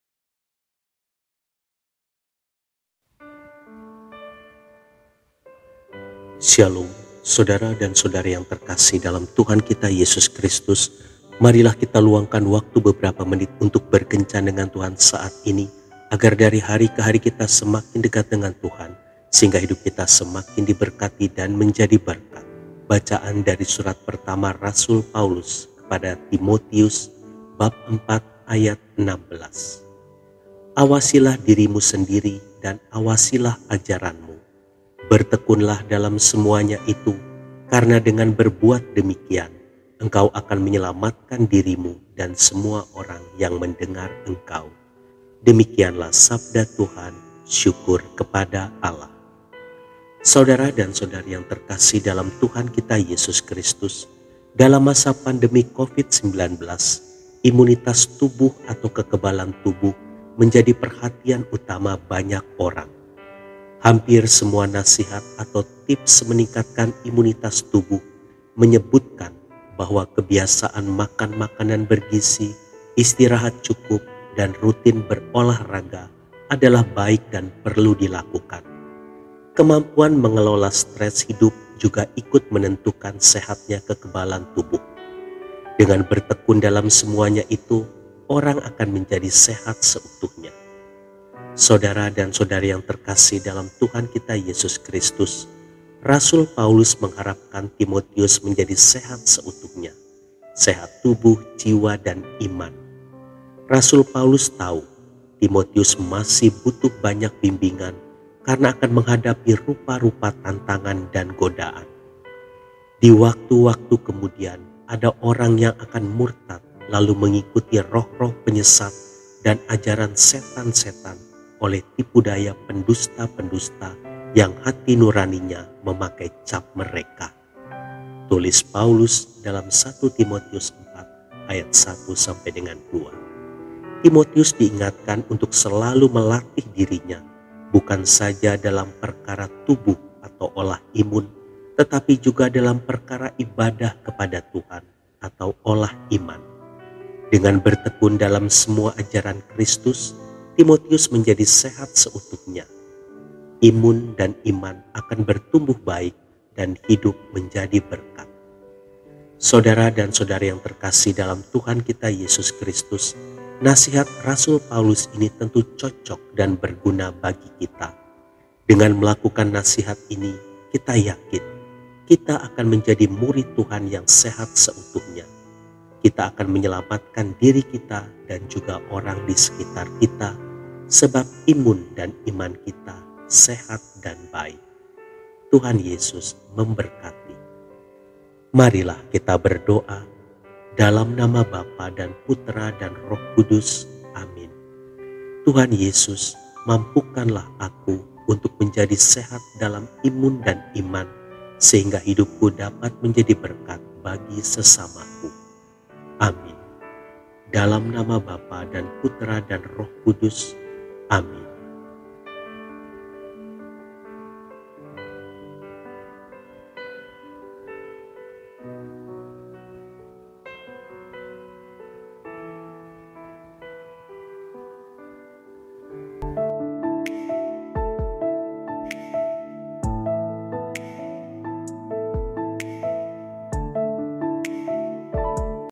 Latihlah diri dalam perkara tubuh (olah imun) dan perkara ibadah kepada Tuhan (olah iman).
1 Timotius 4:16
"Awasilah dirimu sendiri dan awasilah ajaranmu. Bertekunlah dalam semuanya itu, karena dengan berbuat demikian engkau akan menyelamatkan dirimu dan semua orang yang mendengar engkau."

Dalam masa pandemi COVID-19, imunitas tubuh atau kekebalan tubuh menjadi perhatian utama banyak orang.

Hampir semua nasihat atau tips meningkatkan imunitas tubuh menyebutkan bahwa kebiasaan makan makanan bergizi, istirahat cukup, dan rutin berolah raga adalah baik dan perlu dilakukan. Kemampuan mengelola stres hidup juga ikut menentukan sehatnya kekebalan tubuh. Dengan bertekun dalam semuanya itu, orang akan menjadi sehat seutuhnya.

Rasul Paulus mengharapkan Timotius menjadi sehat seutuhnya: sehat tubuh, jiwa, dan iman.

Rasul Paulus tahu, Timotius masih butuh banyak bimbingan karena akan menghadapi rupa-rupa tantangan dan godaan. "Di waktu-waktu kemudian, ada orang yang akan murtad lalu mengikuti roh-roh penyesat dan ajaran setan-setan oleh tipu daya pendusta-pendusta yang hati nuraninya memakai cap mereka," tulis Paulus dalam 1 Timotius 4:1-2.

Timotius diingatkan untuk selalu melatih dirinya, bukan saja dalam perkara tubuh (olah imun), tetapi juga dalam perkara ibadah kepada Tuhan (olah iman). Dengan bertekun dalam semua ajaran Kristus, Timotius menjadi sehat seutuhnya. Imun dan iman akan bertumbuh baik dan hidup menjadi berkat.

Nasihat Rasul Paulus ini tentu cocok dan berguna bagi kita. Dengan melakukan nasihat ini, kita yakin, kita akan menjadi murid Tuhan yang sehat seutuhnya. Kita akan menyelamatkan diri kita dan juga orang di sekitar kita, sebab imun dan iman kita sehat dan baik.

Doa:
Tuhan Yesus, mampukanlah aku untuk menjadi sehat dalam imun dan iman sehingga hidupku dapat menjadi berkat bagi sesamaku. Amin. (Dod).